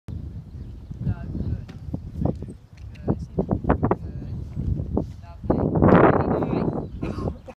Cody at Penny's Gridwork Clinic - 1st video
This is Cody doing Penny Sangsters Gridwork Clinic with the Pony Club on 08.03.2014. It isn't very big though he has improved so much over these last couple of months!

Molly :)